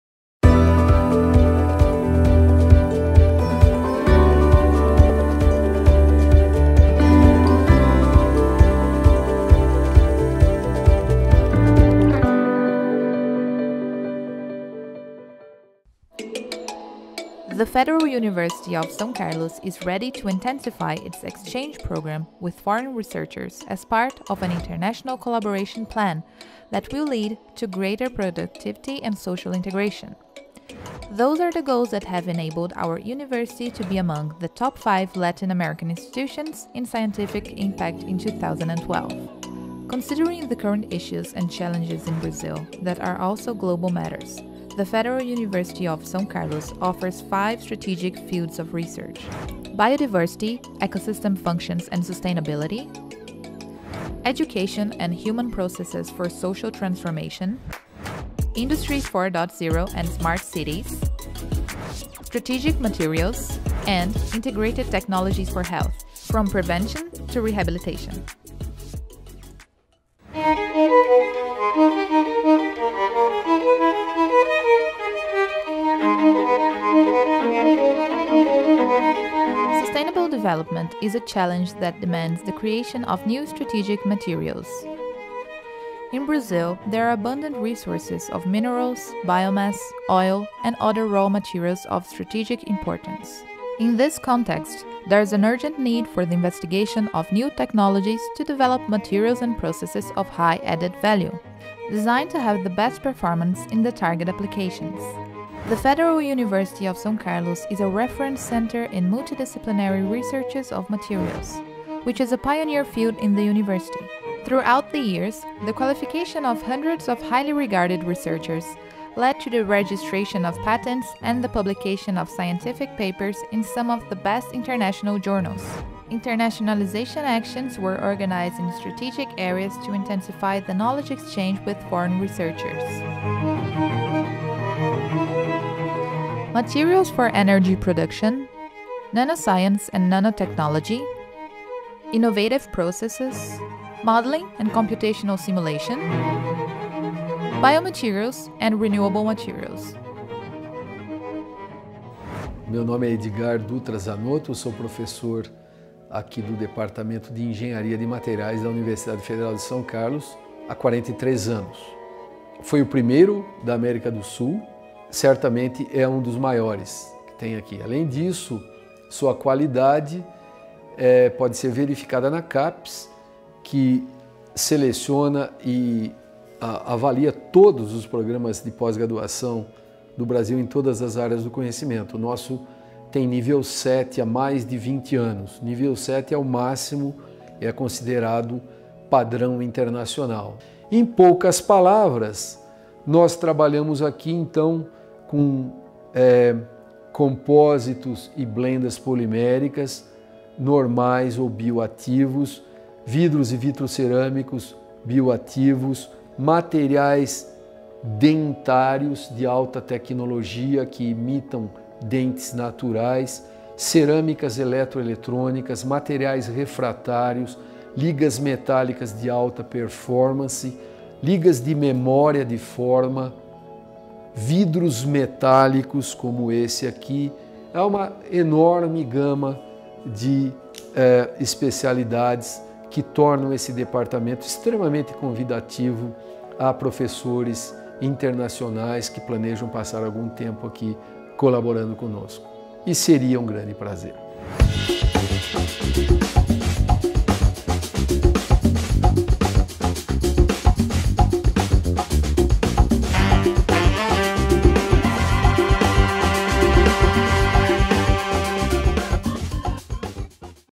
UFSCar PrInt Capes Strategic Materials
The sustainable development of the planet implies a growing demand for new strategic materials. In  Brazil,  there  are  abundant  resources  in  minerals,  biomass,  petroleum  and  other  input  of strategic importance, making even more pressing the prospection for modern technologies for the development of materials and processes of higher added value, designed to obtain the maximum performance in applications of interest. The proposal of this theme is based on UFSCar role in studies involving materials and its multidisciplinary character. In this context, the theme “Strategic Materials” has been chosen as a priority area for the joint action of nine Graduate Programs part of  Exact  and  Technological  Sciences  Centre,  having  3  of  them  score  7  by  CAPES  (PPGQ, PPGCEM, PPGEQ). These features consolidate UFSCar as a Center of Excellence in Materials, with  the  training  of  hundreds  of  masters  and  doctors,  registration  of  dozens  of  patents  and publication of hundreds of international articles in the last four years. It should be mentioned the ability of researchers working on this theme to raise funds and obtain human resources for the development of their researches either from funding agencies as from the private sector, in Brazil and abroad, being some of them CEPID’s (3), thematic FAPESP (3), INCT-CNPq, Petrobras (3), double  major  (Brazil-USA)  and  joint  supervision  (Spain,  Cuba,  etc.).  UFSCar  international recognition  will  be used in  this internationalization proposal to attract  young talents from Brazil and abroad as well as to consolidate current partnership with foreign institutions and acknowledge researchers.  The  internationalization  actions  will  be  directed  in  an  integrated  way  to  promote information stream and knowledge exchange, in the following sub-areas:  Materials for  Energy; Nanoscienceand Nanotechnology; Biomaterials and Renewable Materials; Innovative Processes and Computational Modeling and Simulation